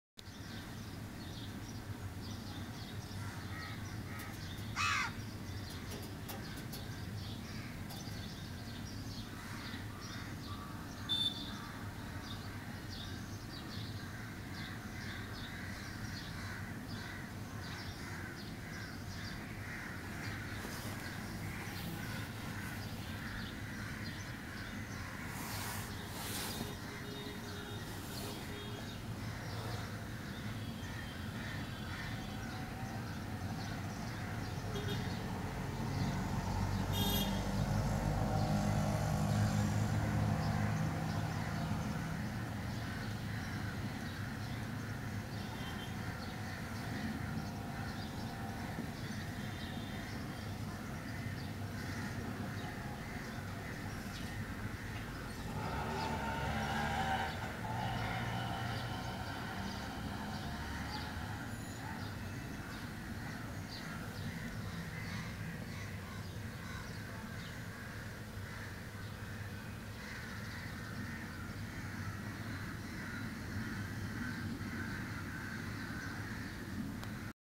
Curious crow
The case of a curious crow & random Mumbai street view... love birds, even though they can be pests...